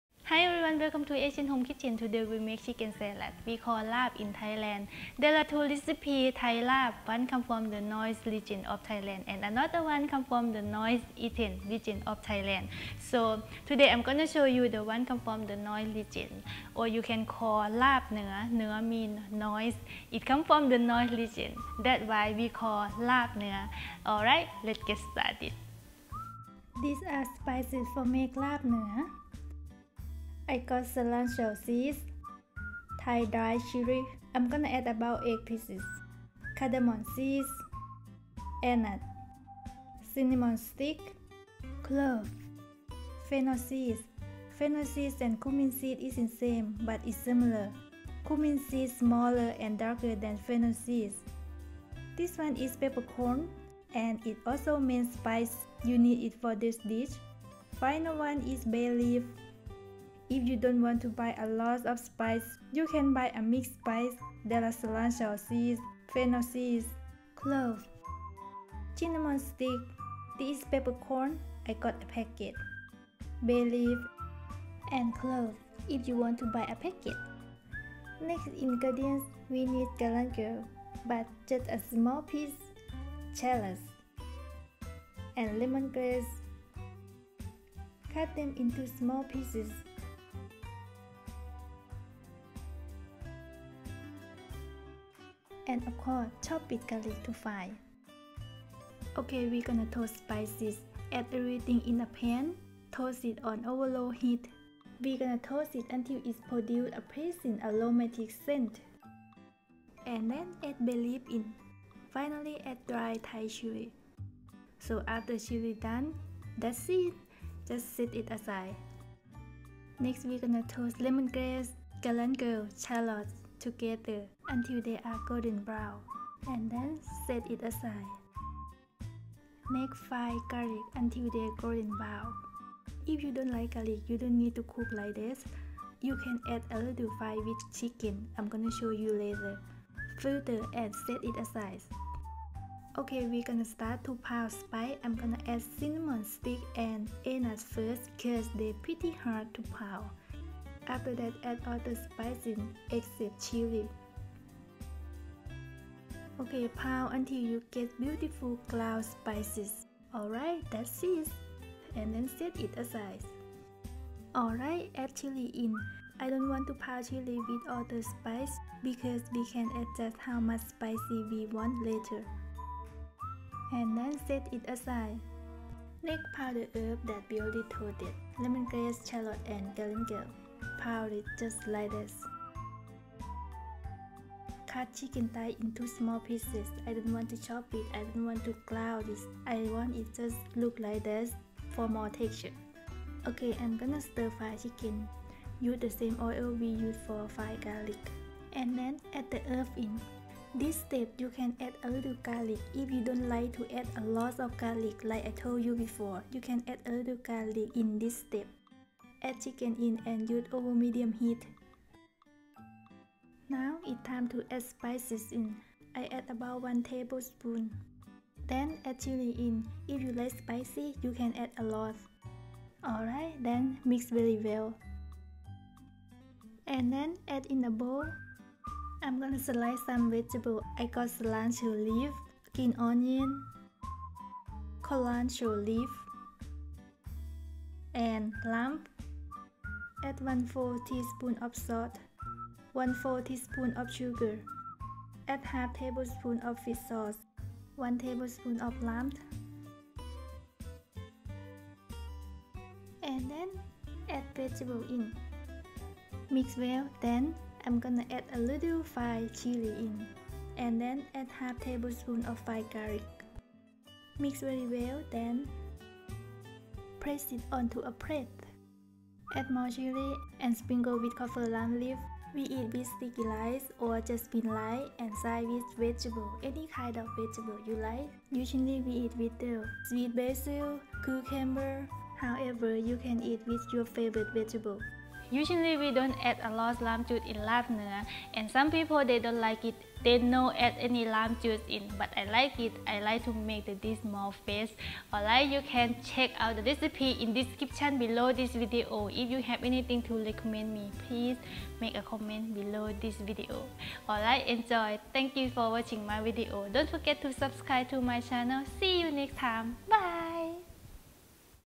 Chicken salad Thai larb Gai recipe ลาบไก่เหนือ
Chicken salad or we called Larb in Thailand. There are two recipe Thai Larb, One come's from the north region of Thailand and another one come's from the northeastern region of Thailand. Today I'm going to show you the one come's from the north region so you can called it Larb Nua(Nua/north)

Ingredients
370g chicken thigh
3 bay leaves
1/2 tsp cilantro seeds
6 cloves
1 tbsp peppercorn
1 anise
1 cinnamon stick
6 cardamon seeds
8-10 Thai dried chili
Pinch fennel seeds
18g lemongrass
25g  shallot
garlic (as needed)
small piece galangal
1/2-1 tbsp fish sauce
1/4 tsp sugar
1/4 tsp salt
1 tbsp lime
kaffir lime leaves
Cilantro
green onion
culantro